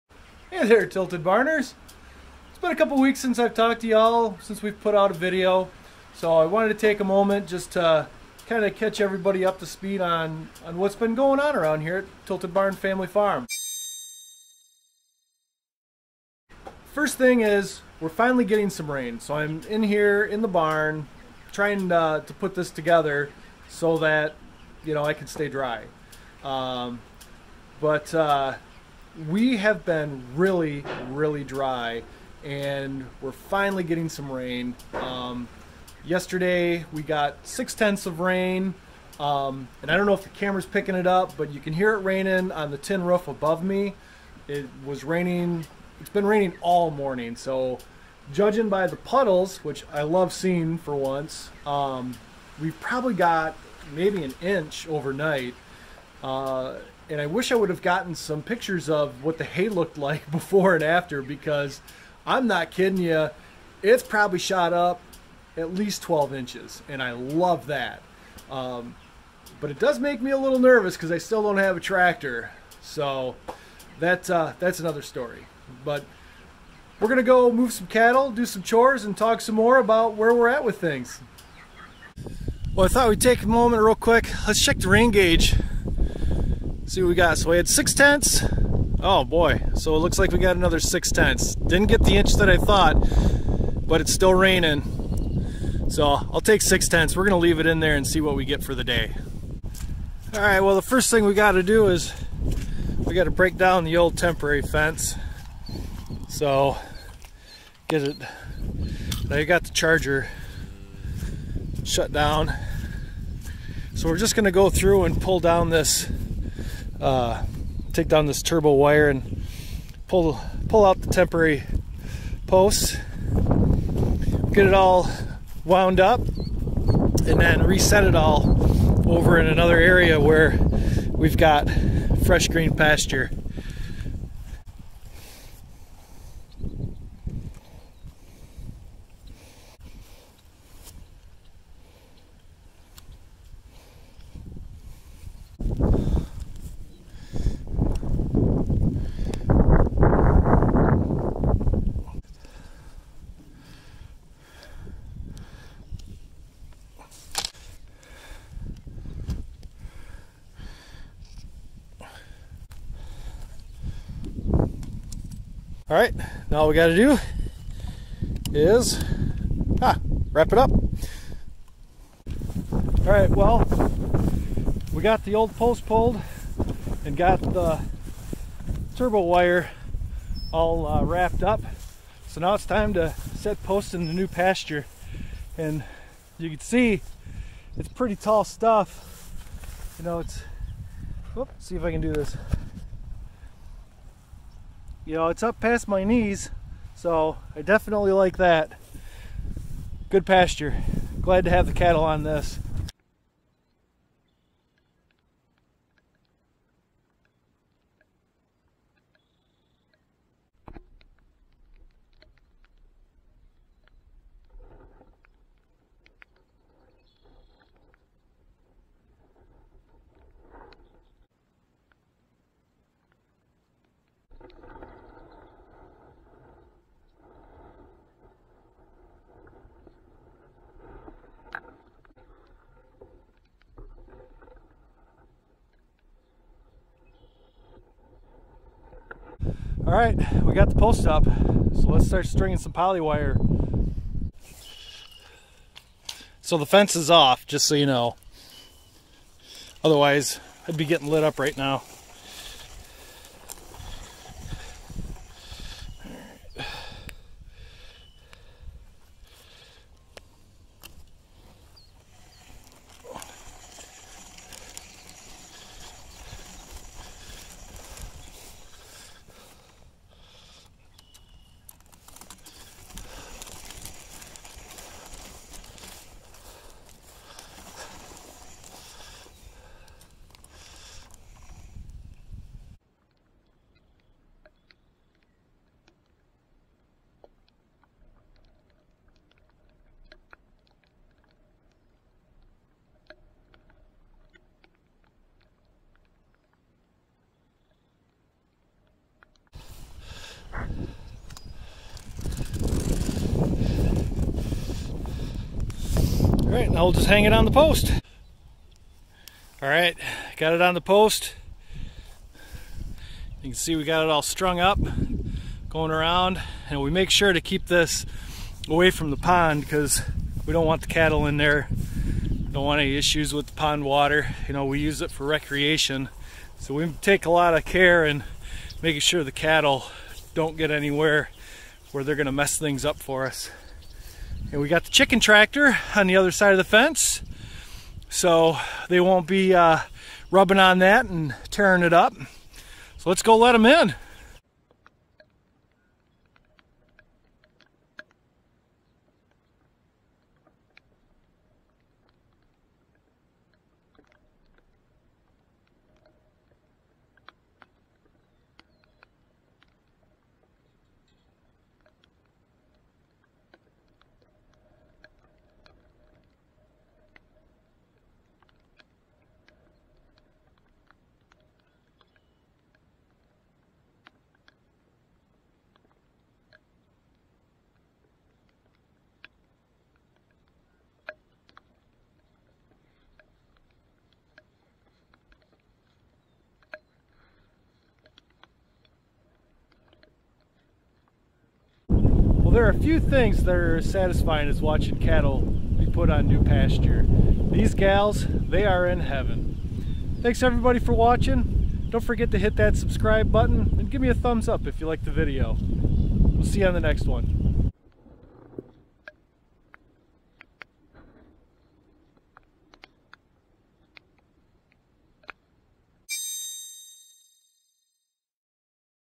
Setting Up Temporary Fencing for Cattle Grazing
We finally get some rain and the pastures are starting to grow.  We go around and set up temporary step in posts for the cattle to graze on the lush grass coming thru.